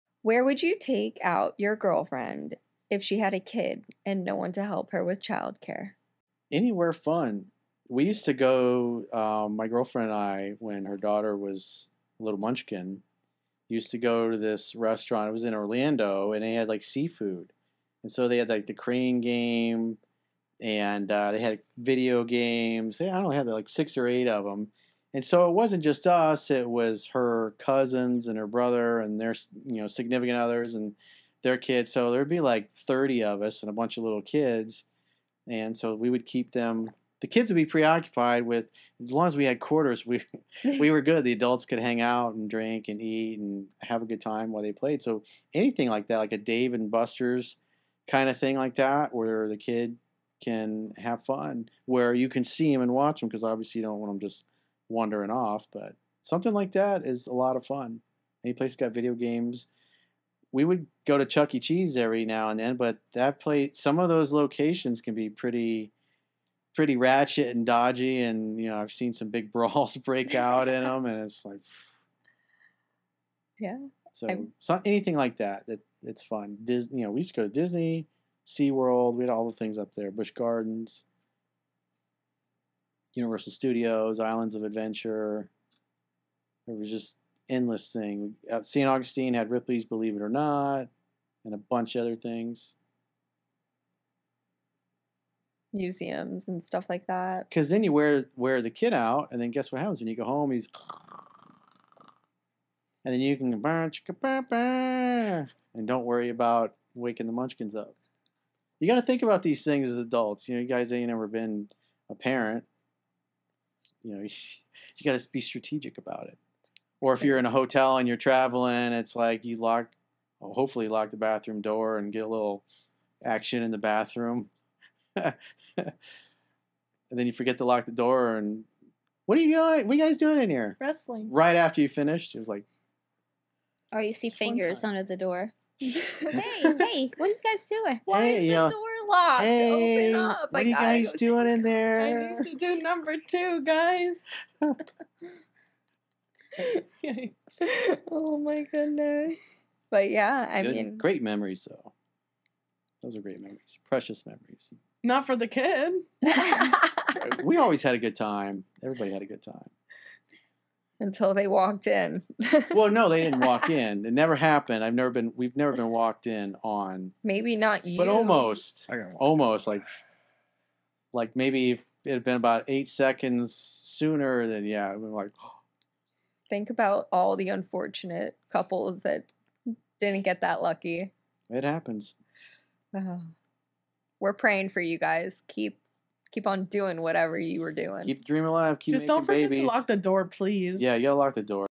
Where Would You Take Your Girlfriend & Her Kid If She Didn't Have Childcare?
Corey, Erica, Jade & Jocelyne discuss a viewer question on where to take your girlfriend & her kid if she didn't have childcare.